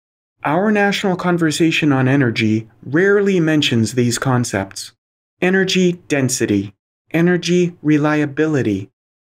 1h57m38s23f Never Mentioned: Energy Density and Energy Reliability - TR2016a
Our national conversation on energy rarely mentions these concepts. Energy density. Energy reliability.